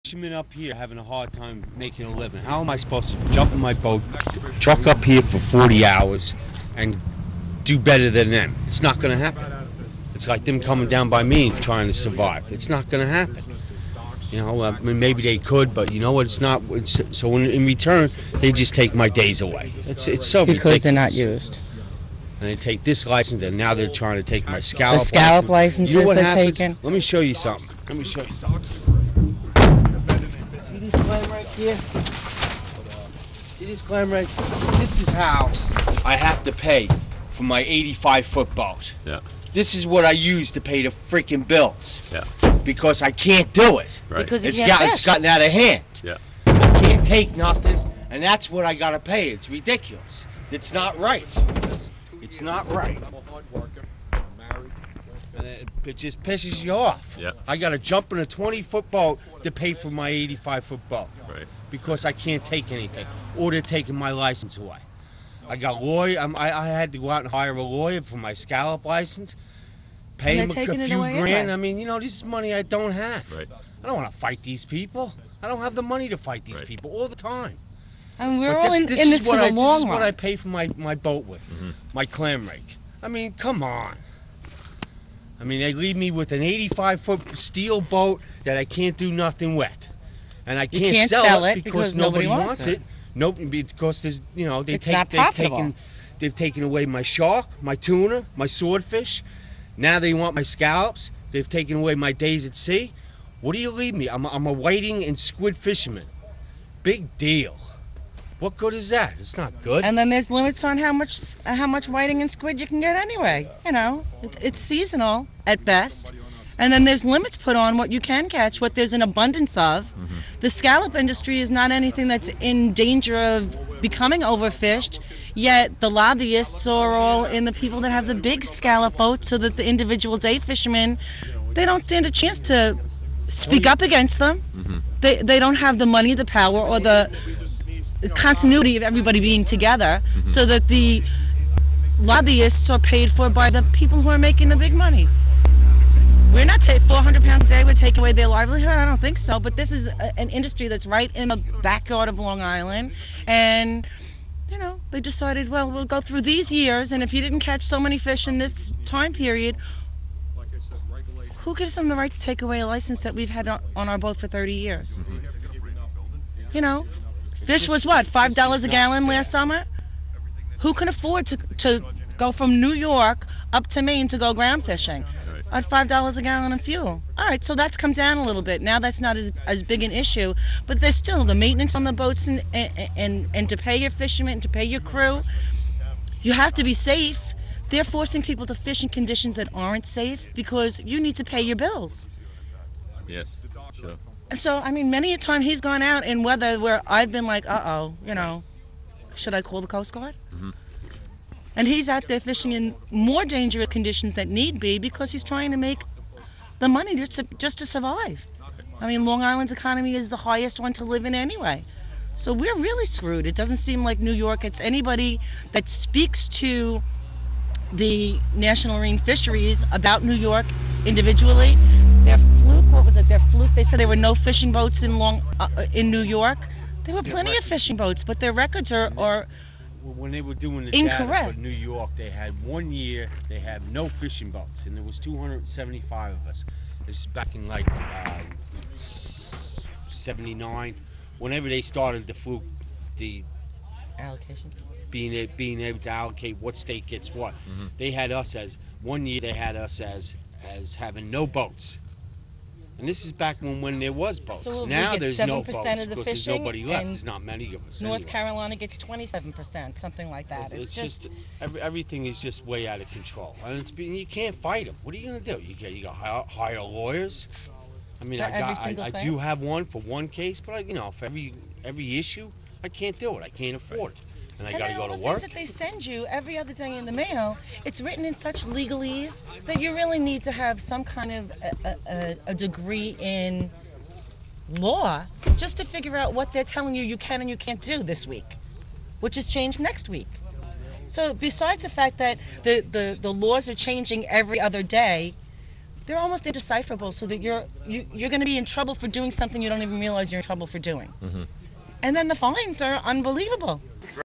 New York Fisherman Speaks Out On N.M.F.S.
Protest in Gloucester.